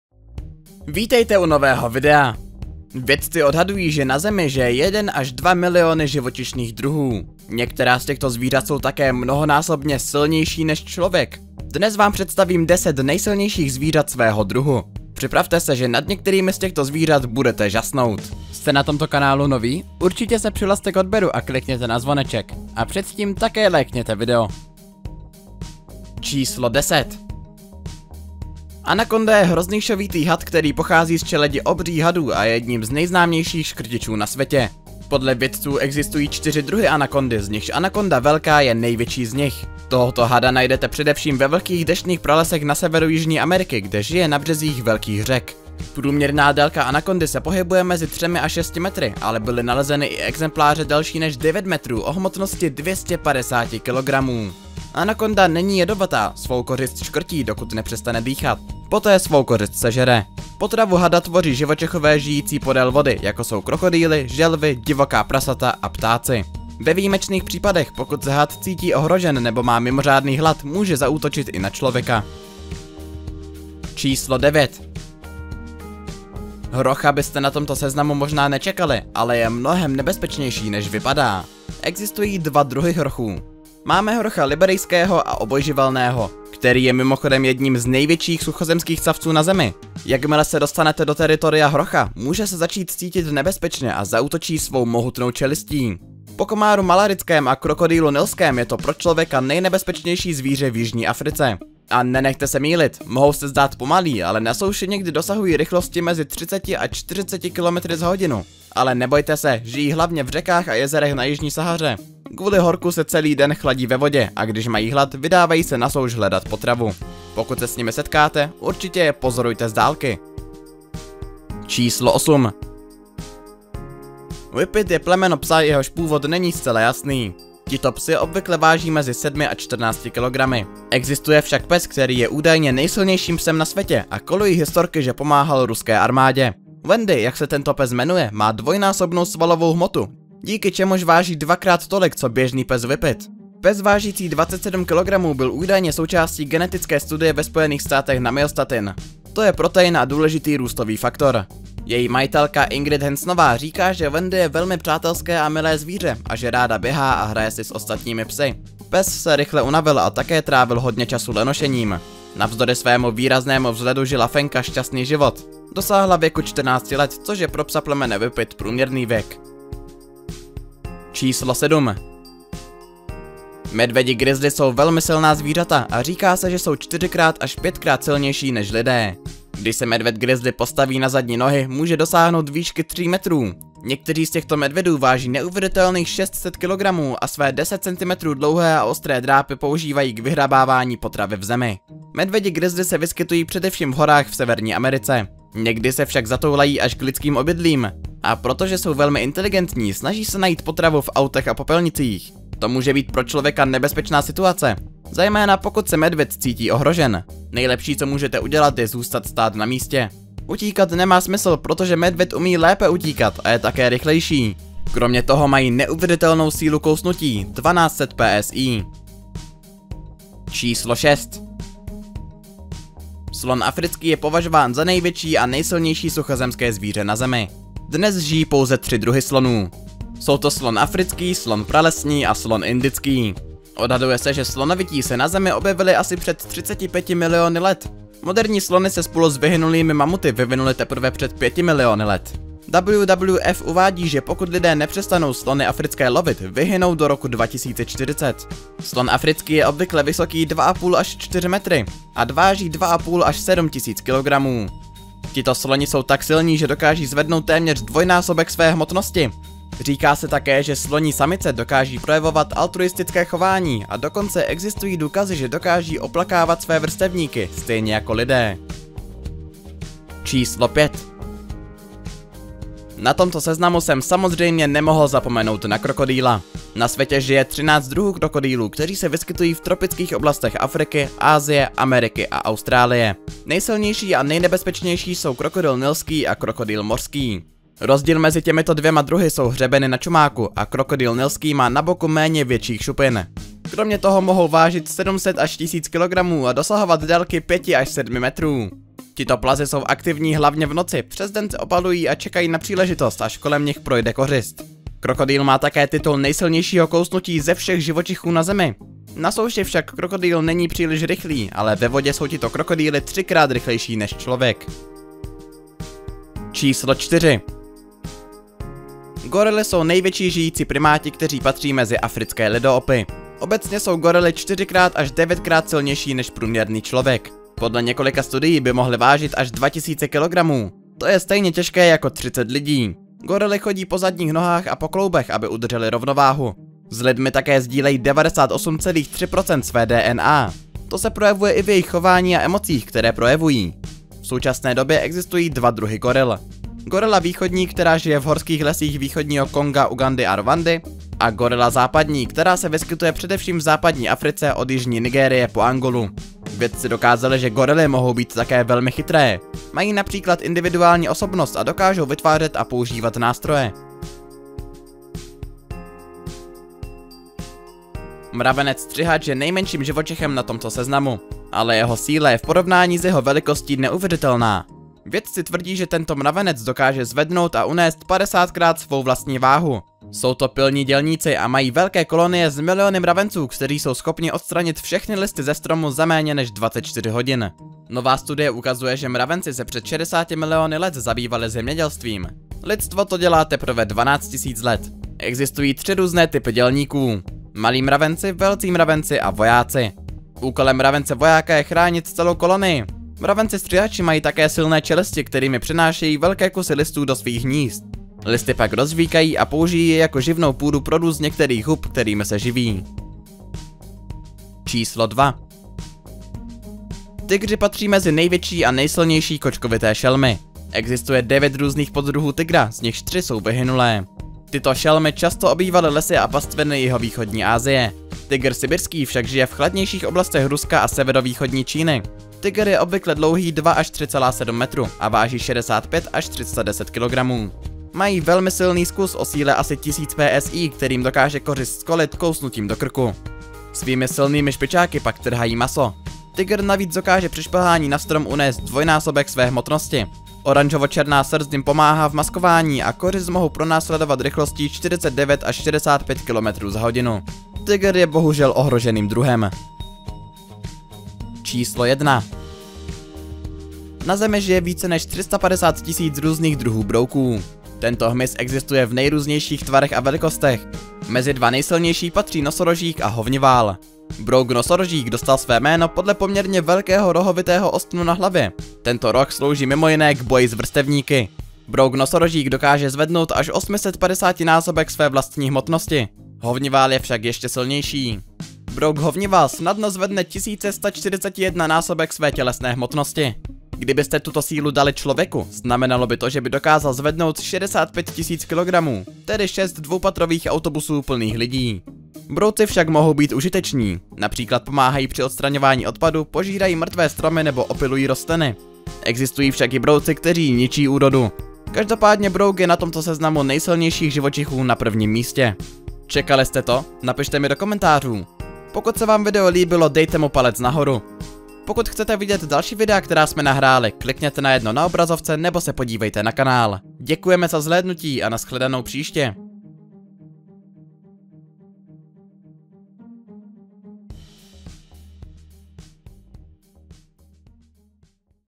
10 Nejsilnějších Zvířat Svého Druhu
Vědci odhadují, že na Zemi žije 1 až 2 miliony živočišných druhů. Některá z těchto zvířat jsou také mnohonásobně silnější než člověk. Od nebezpečných zvířat, jako je had anakonda, který žije v amazonském deštném pralese, až po velké gorily a nejsilnějšího psa, který kdy existoval.
Dnes vám představíme 10 nejsilnějších zvířat svého druhu.

Vítejte na kanále Fakta Pana Chobotňáka. Zveřejňujeme Top 10 videa na nejrůznější témata založená na faktech, fámách a spekulacích.